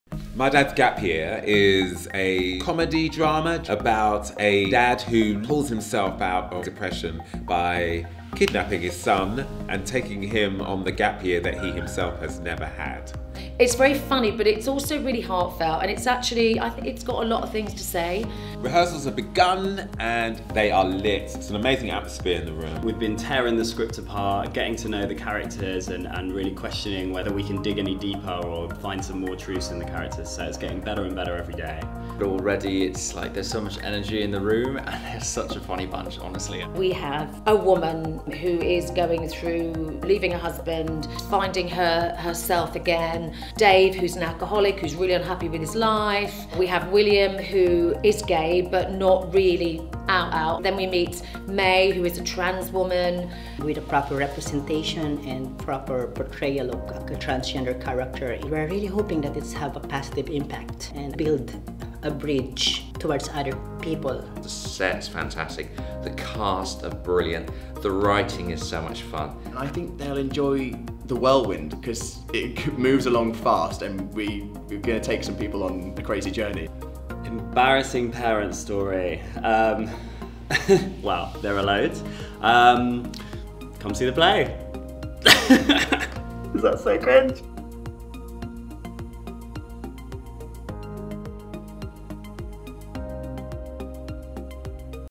My Dad's Gap Year in Rehearsals
My Dad's Gap Year
By Tom Wright, Directed by Rikki Beadle Blair
30 Jan - 23 Feb 2019

“Seriously William, you’re eighteen and you’re gay, for Christ’s sake. You’re meant to swim against the tide. A proper gap year is all about traveling. Seeing the world. Popping your cherry. This is our chance to start living. Me and you.”

This is the story of Dave; a dad in mid-life freefall who takes his repressed, gay, teenage son William on a wild adventure to Thailand to lose his virginity. Written by Tom Wright and directed by Rikki Beadle-Blair (MBE), My Dad’s Gap Year is a wickedly funny and heartfelt story based on true events.

Starring Michelle Collins (Eastenders, Coronation Street) as William’s mother Cath – a woman on her own journey of self-discovery – alongside Adam Lannon, Alex Britt, Victoria Gigante and Max Percy.

Gay love, straight love, trans love, buddy-love, drinking games and beer bellies. Fasten your seat belts, it’s gonna be a mad one!